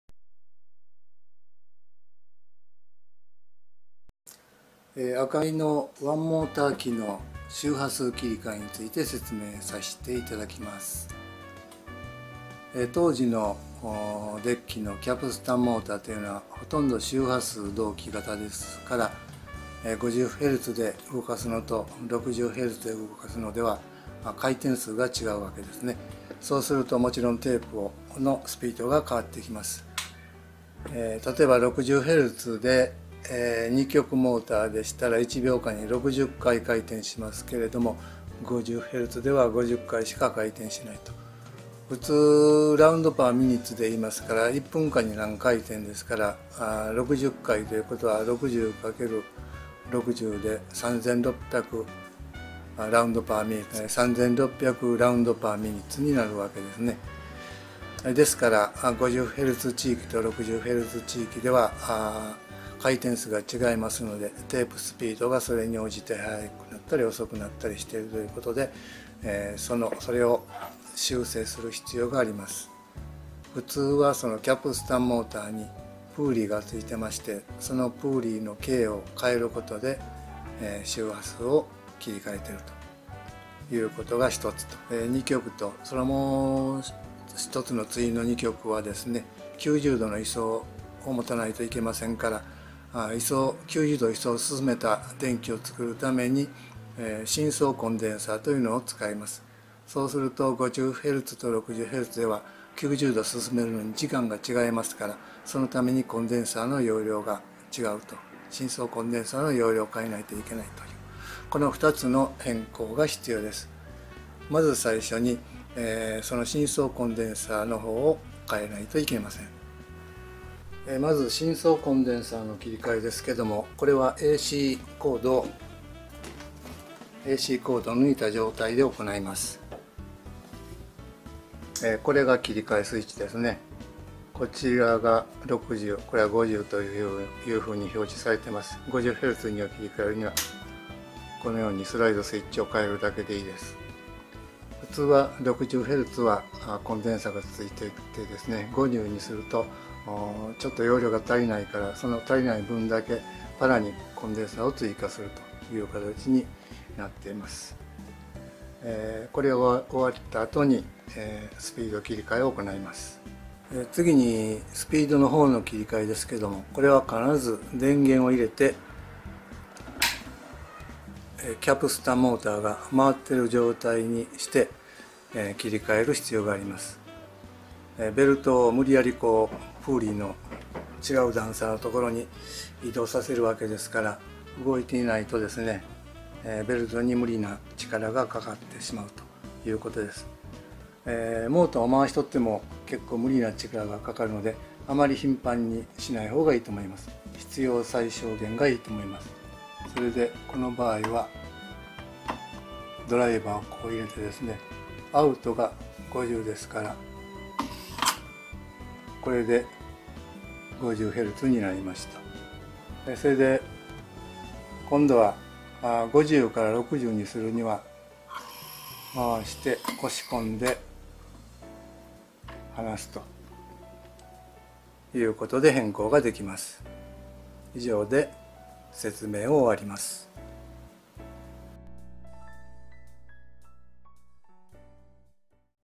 AKAI ワンモーターオープンリールデッキの電源周波数切り替え方法
4000シリーズ4440シリーズのようなワンモーター機の電源周波数切り替えはサービス対応ではなく、ユーザーコントロールとなってます。
自分で切り替え可能ですのでその方法のビデオを作りました。